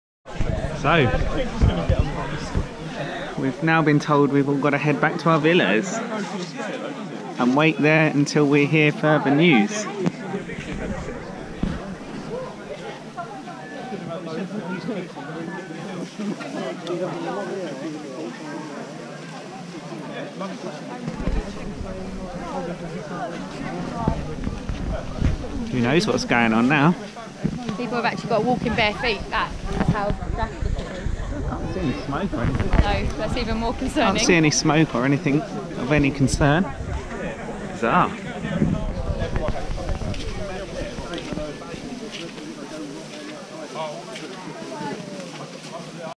Centre Parcs Elvedon Evacuation!!!!!!! 23rd July 2017
Bomb scare at Centre Parcs Elvedon!!! 23rd July 2017 total chaos and confusion at a trusted family resort!